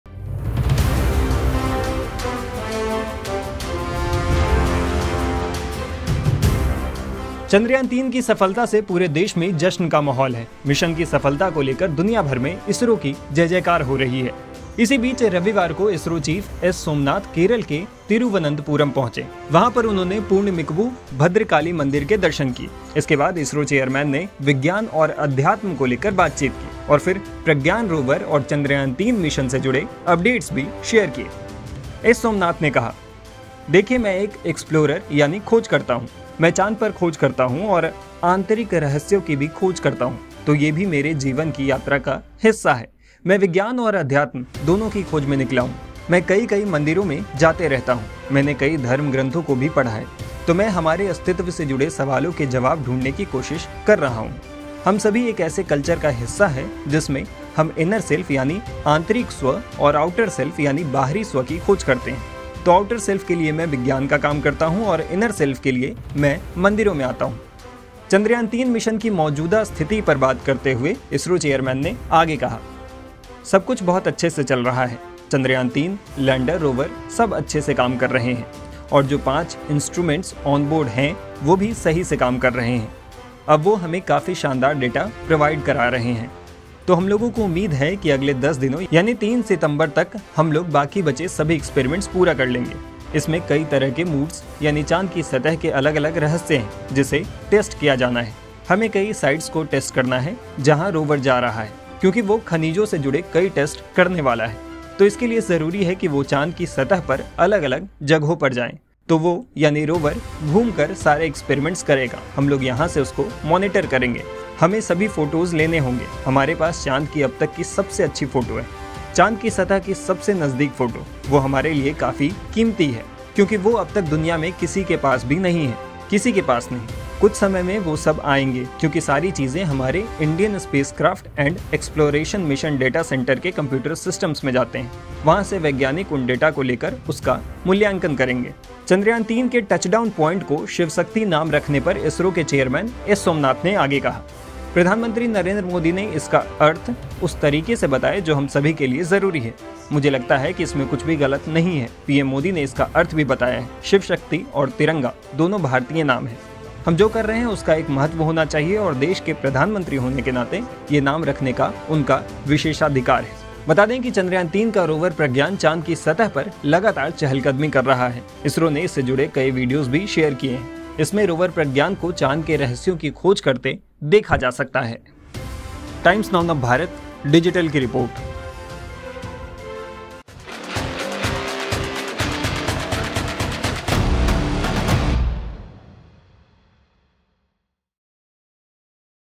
Chandrayaan 3, Rover Pragyan और मंदिर जाने पर क्या बोले ISRO Chief S Somanath ? | Hindi News
चंद्रयान-3 की सफलता से पूरा देश में जश्न का माहौल है. मिशन की सफलता को लेकर दुनियाभर में इसरो का जय-जयकार हो रही है. इसी बीच रविवार को इसरो चीफ एस. सोमनाथ केरल के तिरुवनंतपुरम पहुंचे. वहां पर उन्होंने पूर्णमिकवु, भद्रकाली मंदिर के दर्शन किए.